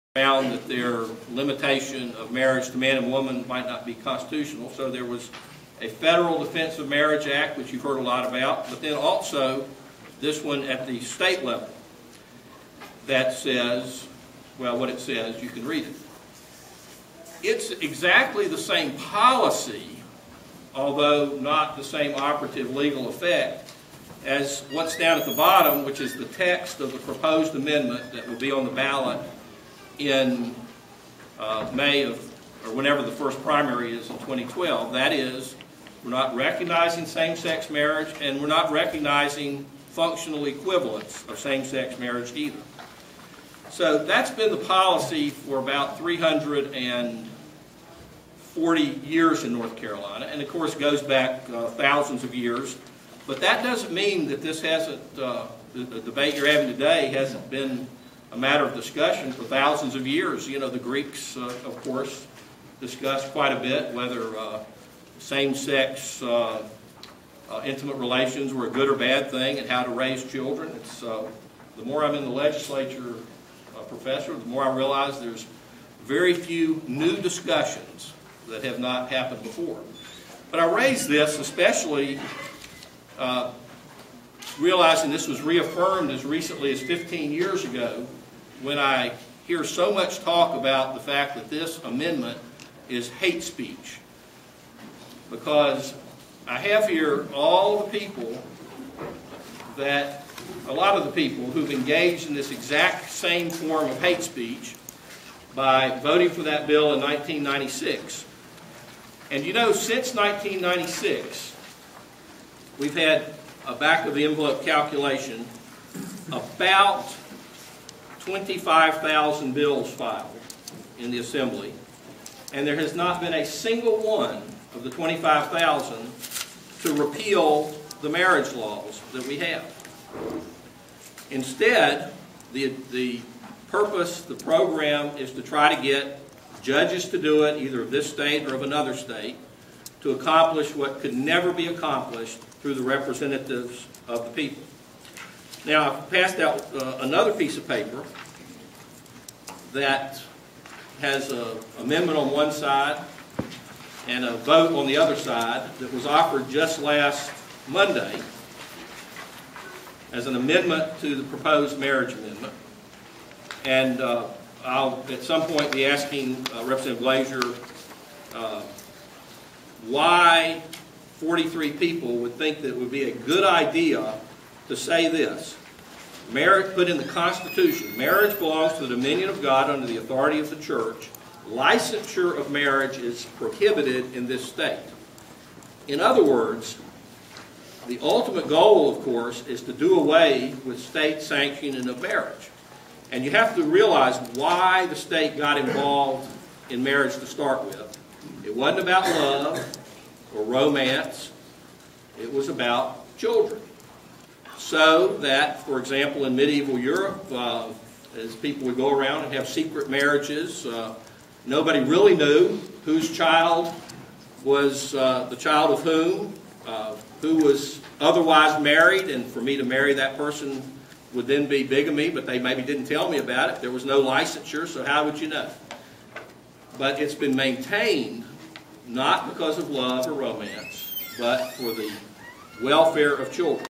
NC State House Majority Leader Rep. Paul 'Skip' Stam on NC Marriage Amendment
Stam has an interesting fixation on polygamy, bigamy and adult incest and wants to see NC to